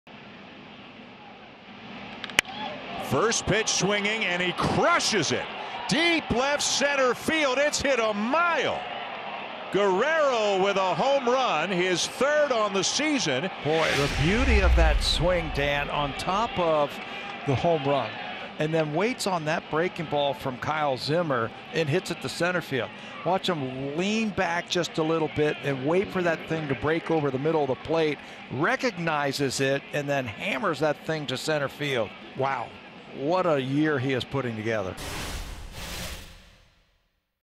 Vladimir Guerrero Jr. 456-foot Home Run: 4/15/2021
Statcast measures the exit velocity and projected distance of Vladimir Guerrero Jr.'s 456-foot home run against the Royals.